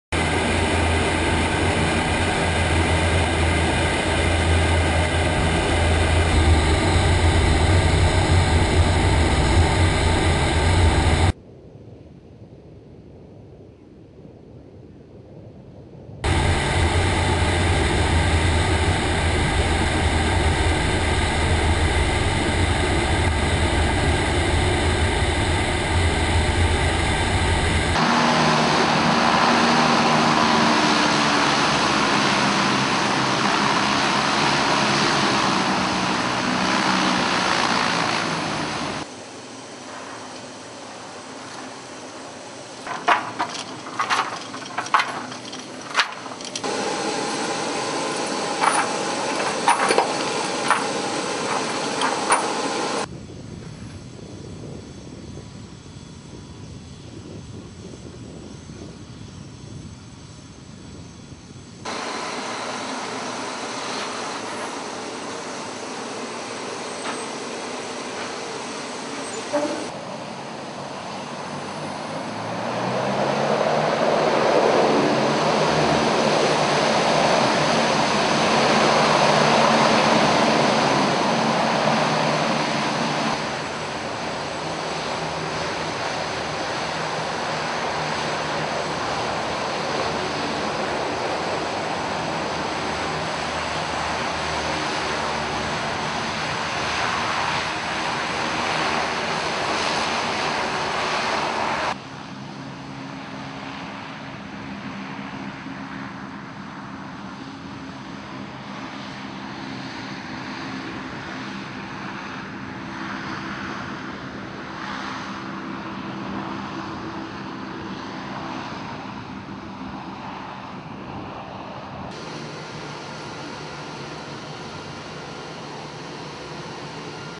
LCAC participates in RIMPAC 2012
Landing Crafts, Air Cushioned (LCAC) departs USS Essex (LHD) 2 and deploys U.S. Marines and equipment to the beach during Rim of the Pacific (RIMPAC) 2012.